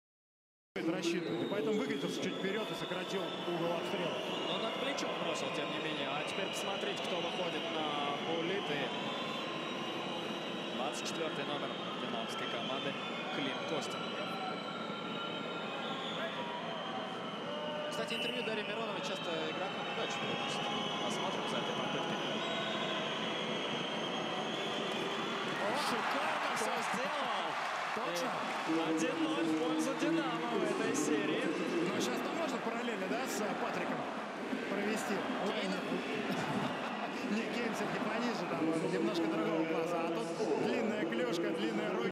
Rookie Klim Kostin magnificient SO goal
Dynamo's rookie forward Klim Kostin (1999) brings additional point to his team at Moscow Derby, scoring perfect SO goal